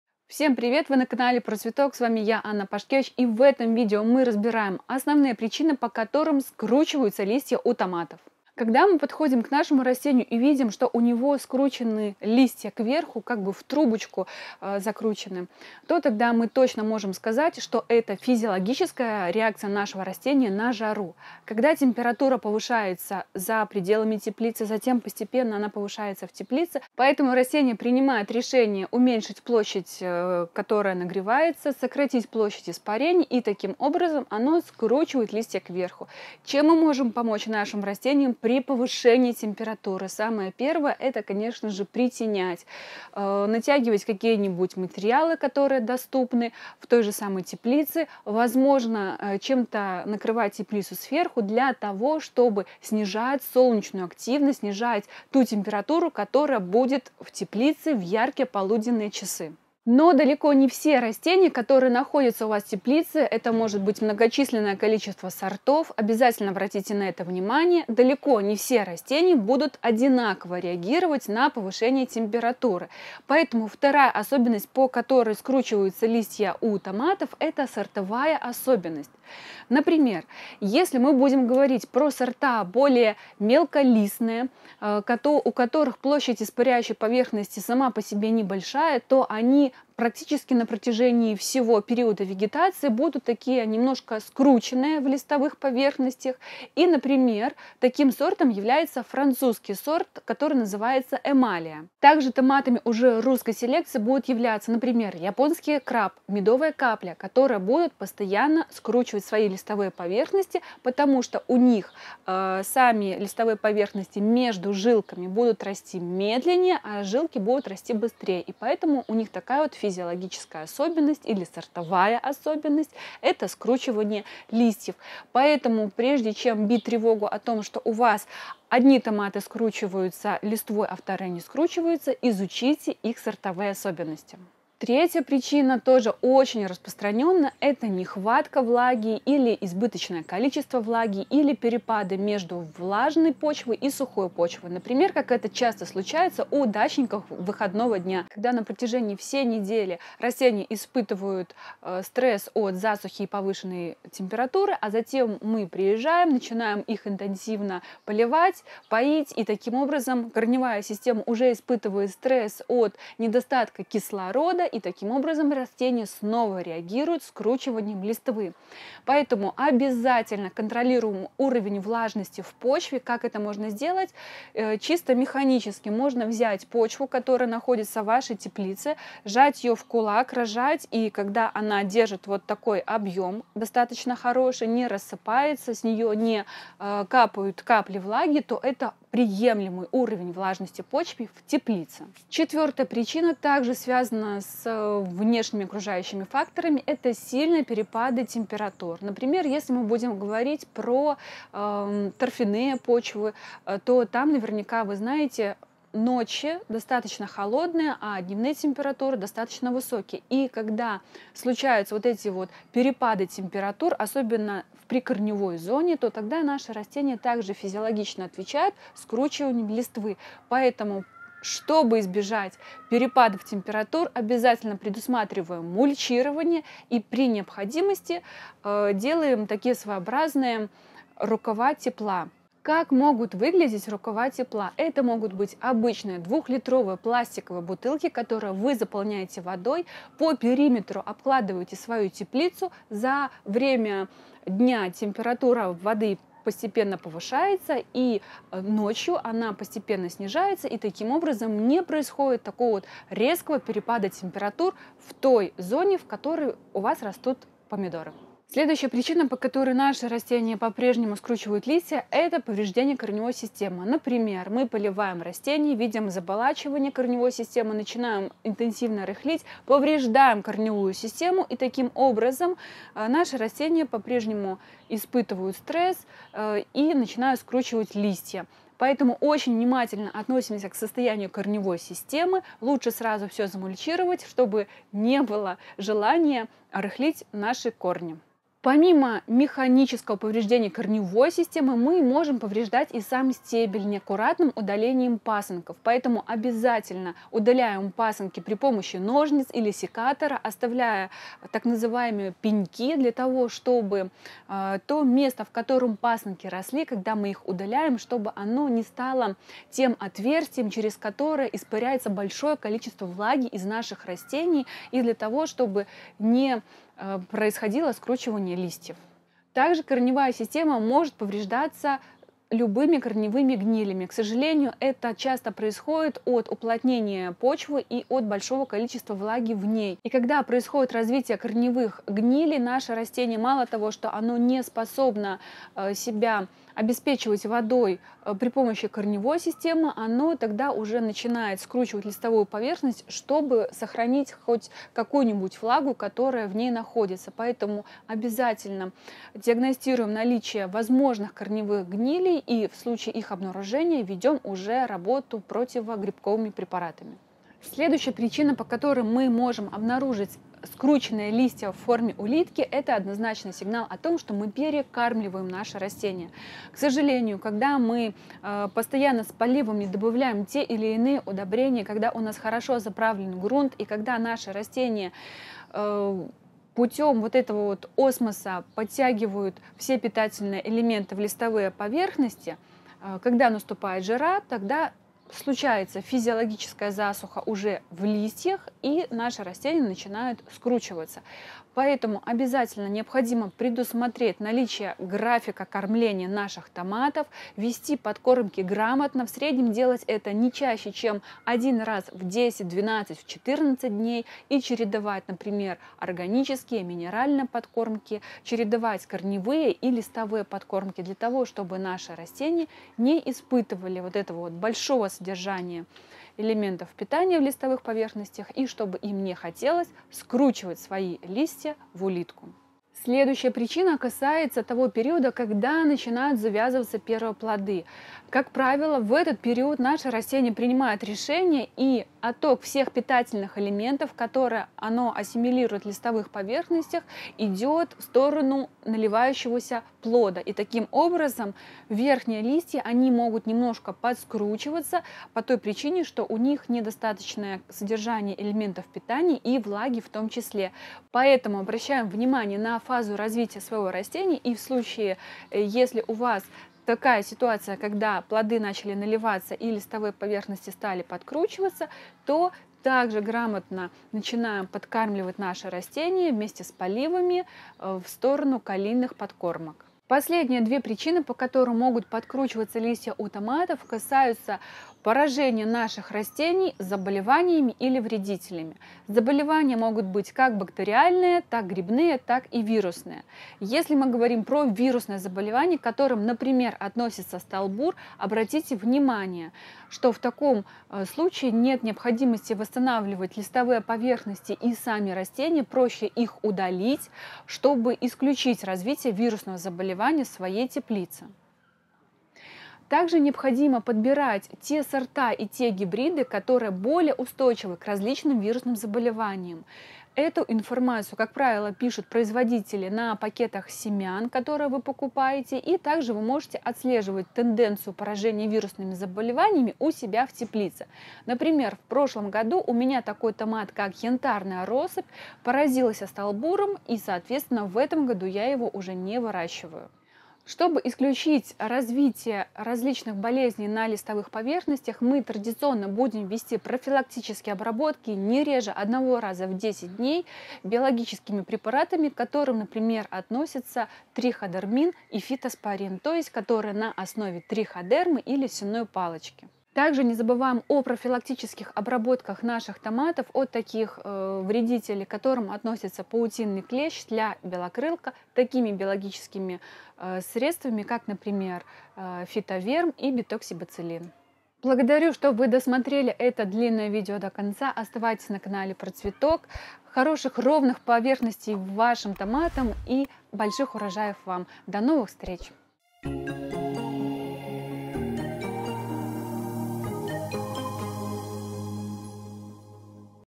РАСКРЫТА ТАЙНА СКРУЧЕННЫХ ЛИСТЬЕВ ТОМАТОВ! Устраните эти причины, чтобы не лишиться урожая!
Почему скручиваются листья у томатов? На то есть целый ряд причин: погодные условия, повреждения корней, неправильный полив, заболевания и даже сортовые особенности. 10 основных причин скручивания листьев томатов в этом видео разбирает биолог Анна Пашкевич и предлагает способы их устранения.

00:00 В этом видео
00:08 Причина 1. Жаркая погода
00:36 Меры при высокой температуре
01:00 Причина 2. Сортовая особенность
02:14 Причина 3. Неправильный водный режим
02:49 Контролируем влажность почвы
03:12 Причина 4. Перепады температур
03:52 Как сделать "рукава тепла" и для чего
04:18 Причина 5. Механические повреждения корней
04:48 Причина 6. Повреждение стебля при пасынковании
05:19 Причина 7. Заболевания корней, гнили
05:57 Причина 8. Жирование
07:02 Причина 9. Активное плодоношение
07:50 Причина 10. Заболевания и вредители
09:24 Заключение